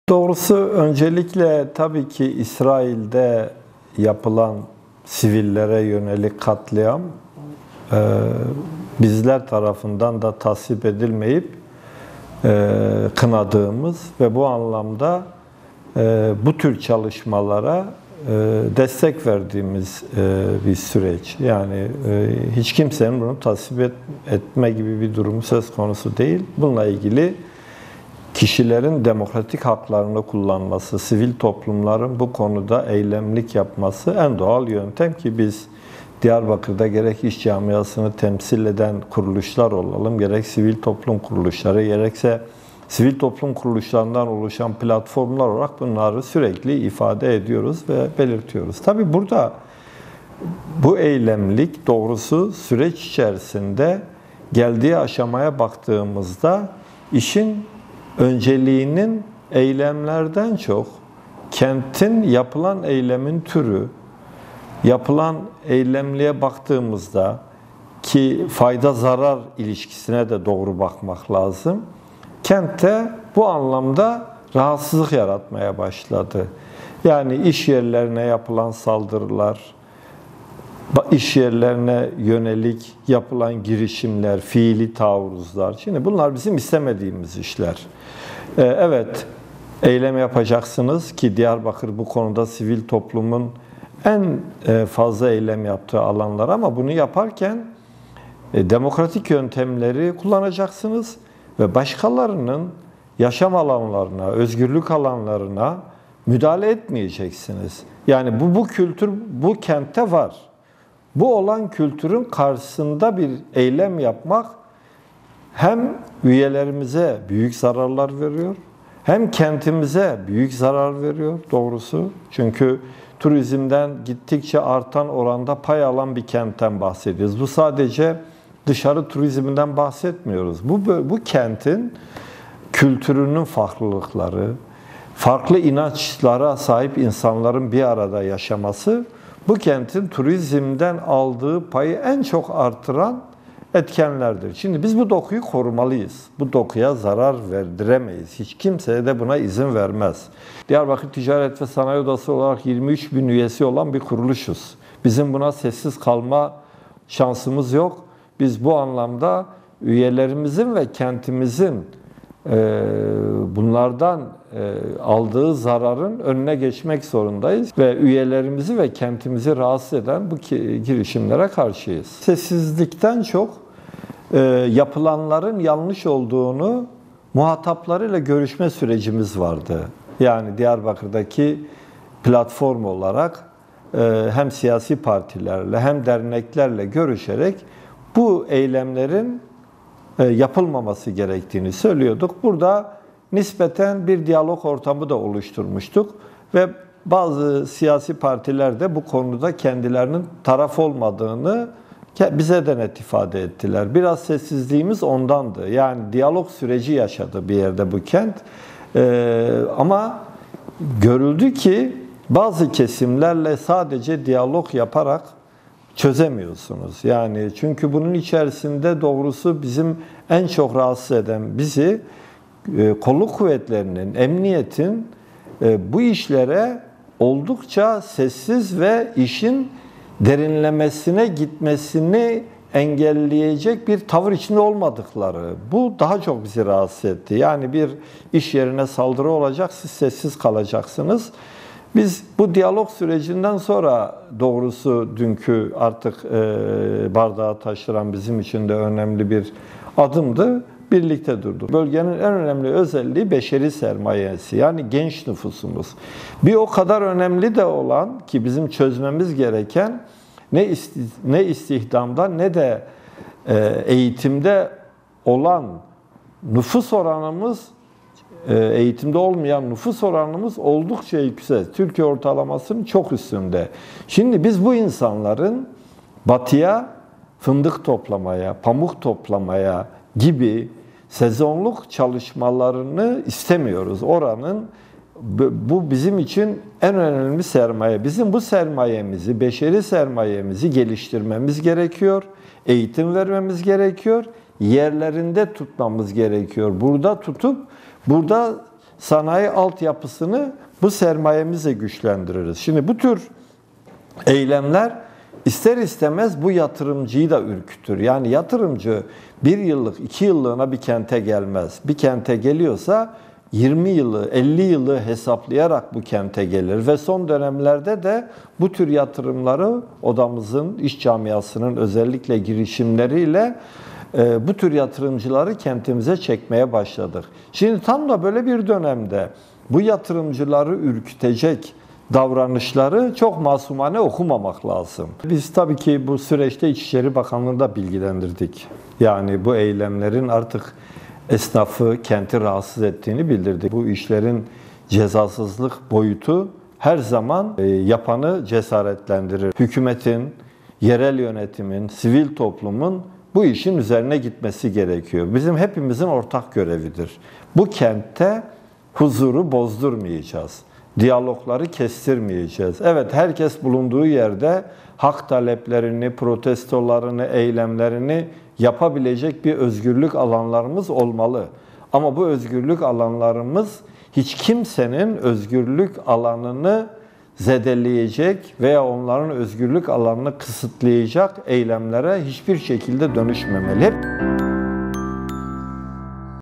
DTSO BAŞKANI MEHMET KAYA: SALDIRILARIN ÜZERİNE GİDİLMELİ
HÜDA PAR'lı grupların saldırılarına tepki gösteren DTSO Başkanı Mehmet Kaya, "Hükümet, yerel yönetim ve sivil toplum bu işin üzerine gitmeli. Bu kentte huzuru bozdurmayacağız" dedi.